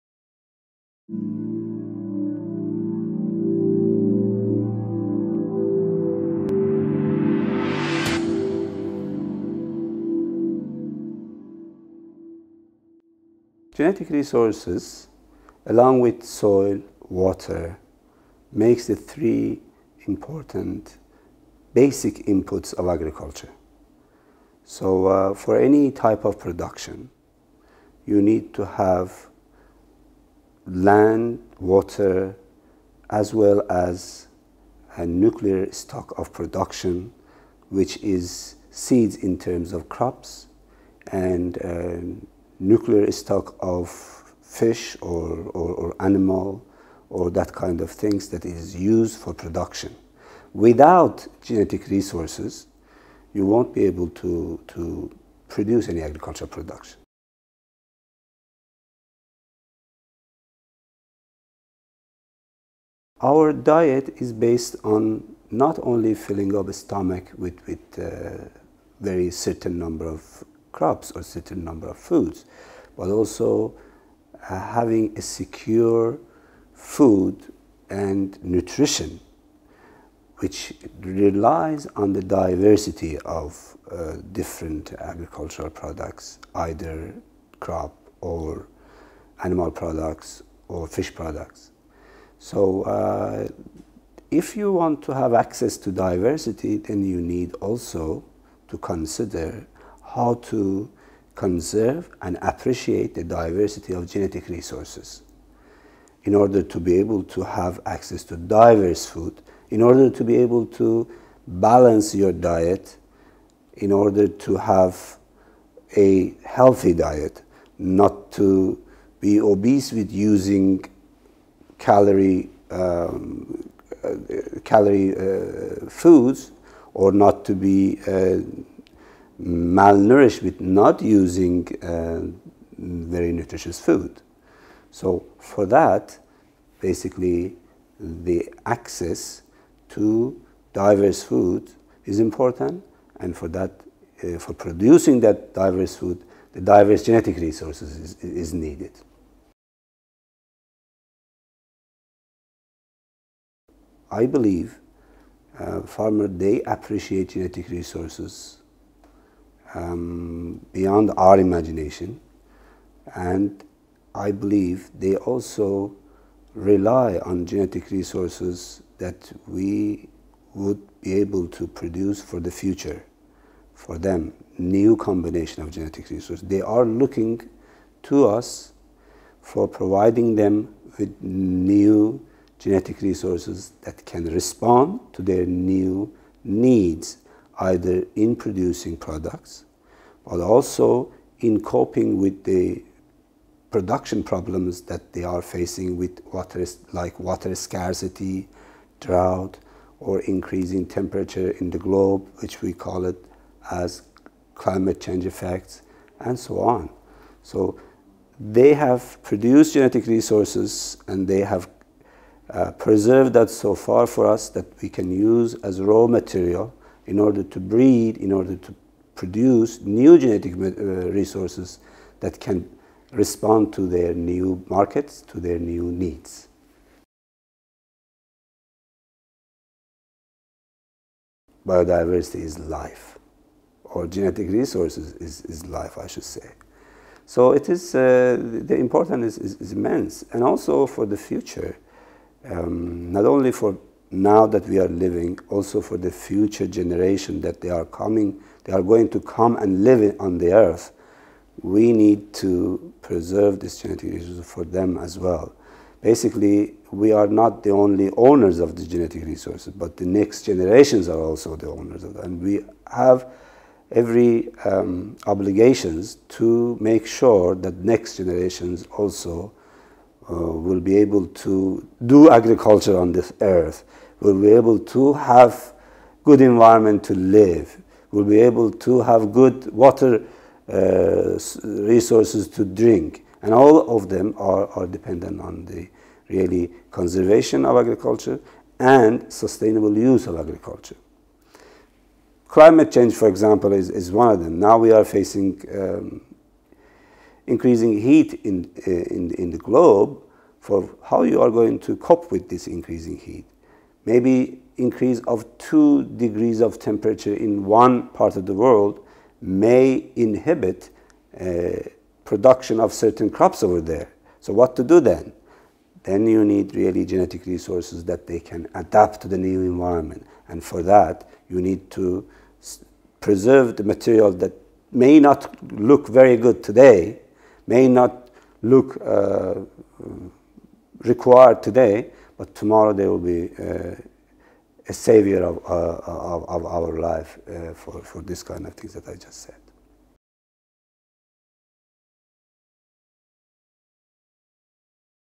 Future generations are the owners of our genetic resources -- J. Mozafari
Javad Mozafari is the Director of the Iranian National Plant Gene-Bank and was the Chair of the Commission's Twelfth Regular Session. Mr. Mozafari emphasizes that diversity of genetic resources and access to diverse food are crucial for a healthy diet. Furthermore, he stresses the needs of the farmers and future generations.